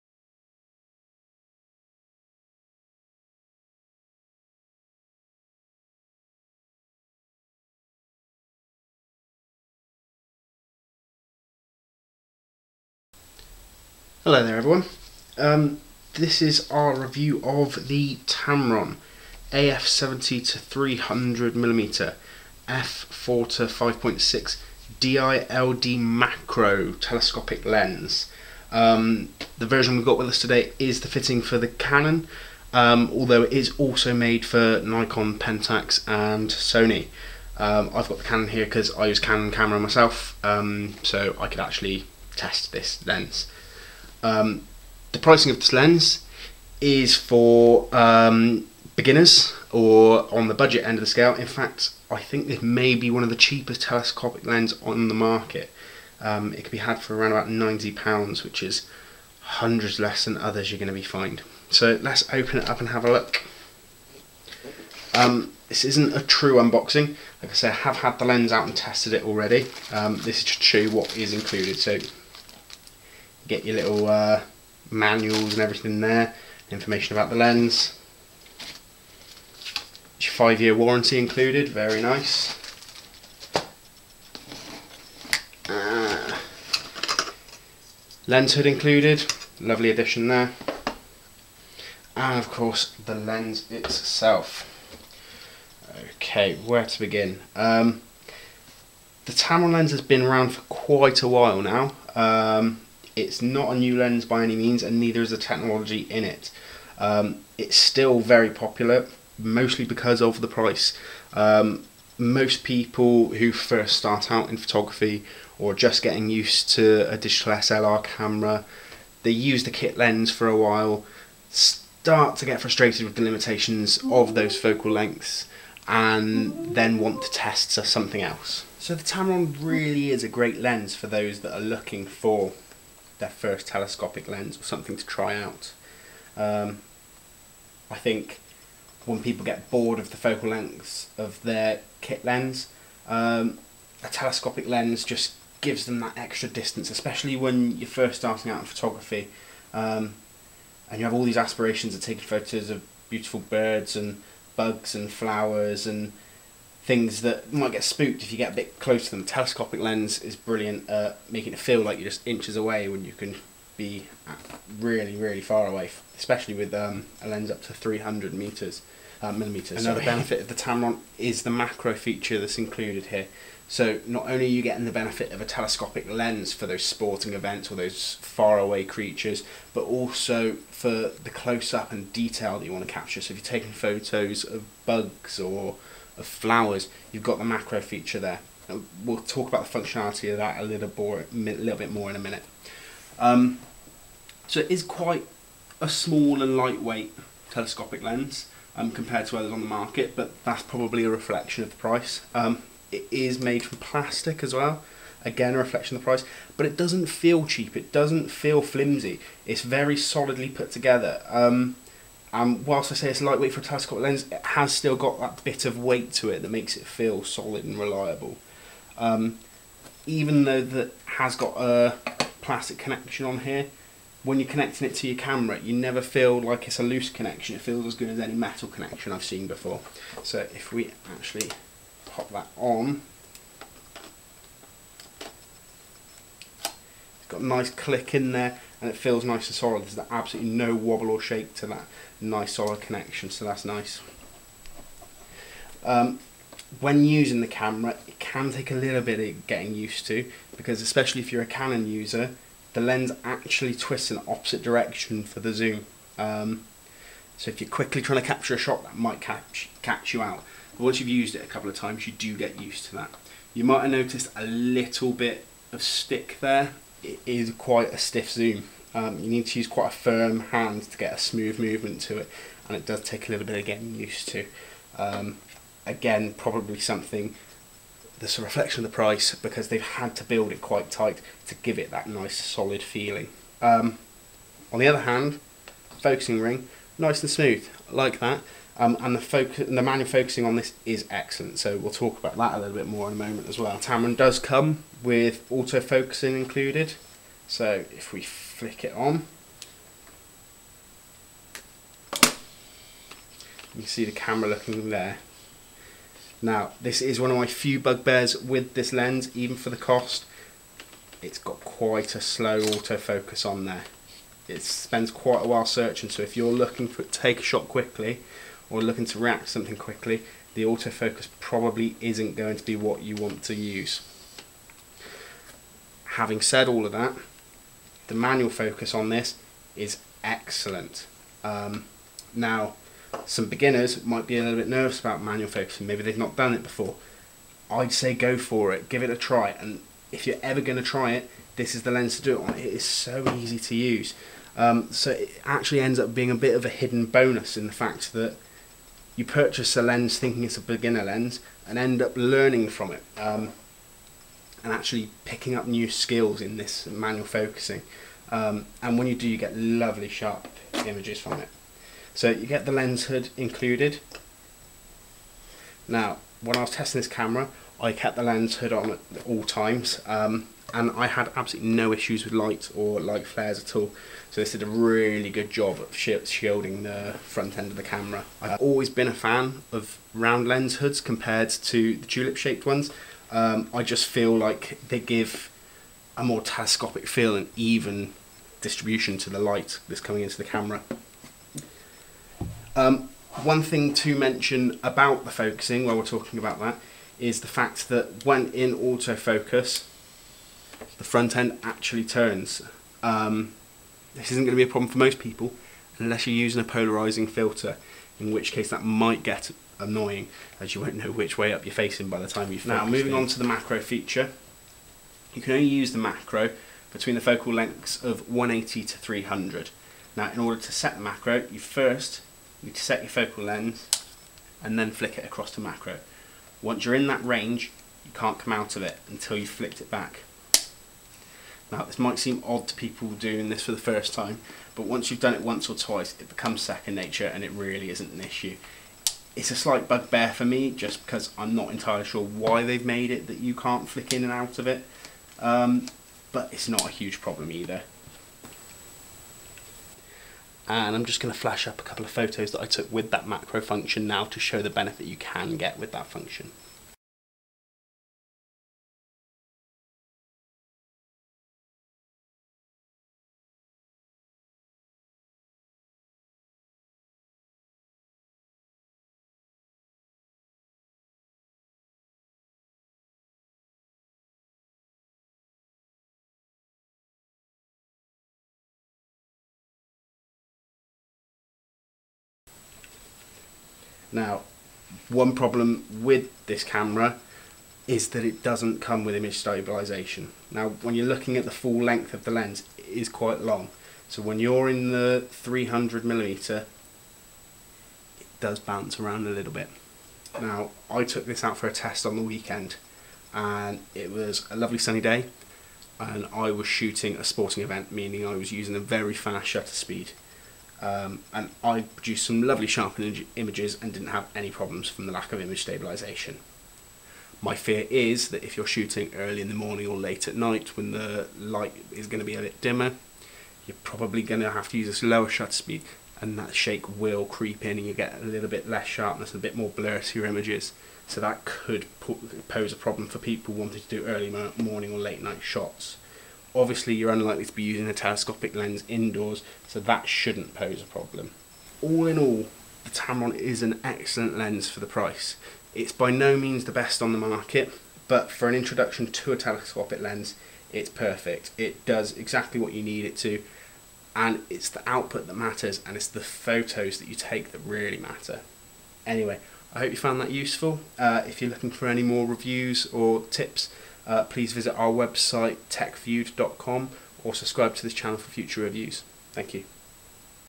Tamron AF 70-300m f/4-5.6 Di LD Macro Lens Review
This is our hands on review of the Tamron AF 70-300m f/4-5.6 Di LD Macro.

To read the full written review and find other tech review, please visit our